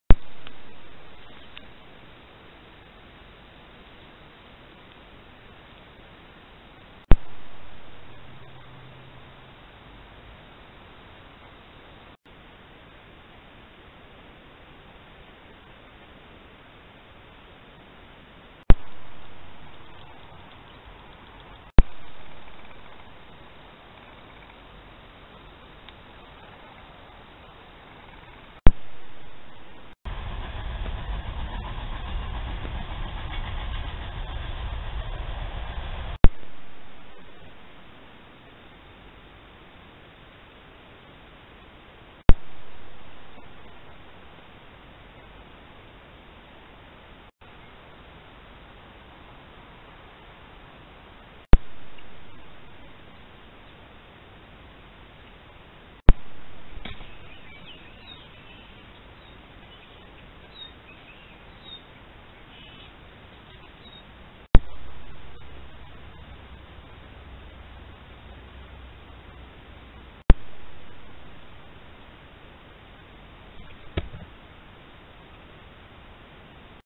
Down at the Lodge
For this week’s TaptealTuesdy we’ve put together a compilation of trail camera footage that we are calling: Down at the Lodge. Some are visitors, some are workers, some come at night and some come at day. The sign-in at the lodge includes beaver, muskrat, mink, raccoon, cottontail rabbit, black-tailed jackrabbit, coyote, black-crowned night heron, northern flicker, Wilson’s snipe, Virginia rail, white-crowned sparrow, mourning dove and magpie.